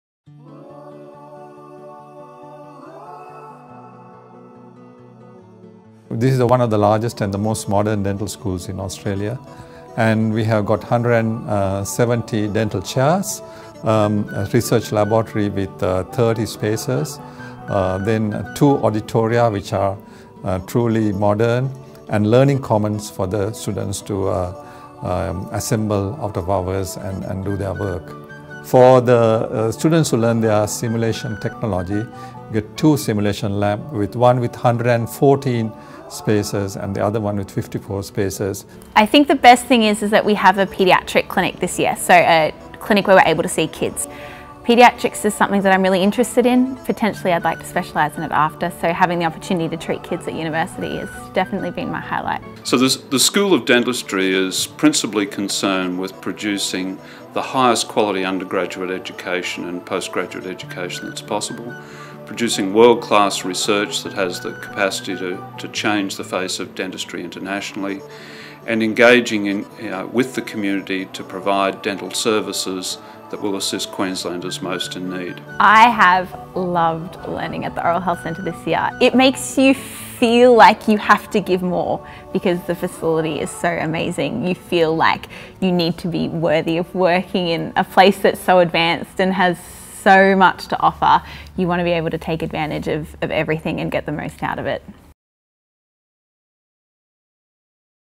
UQ Oral Health Centre
The UQOHC is the nation’s largest and most advanced tertiary oral health facility and brings together renowned academics and researchers, leading practitioners and top tier students who are united by their passion to improve the lives of everyday Australians through better dental care.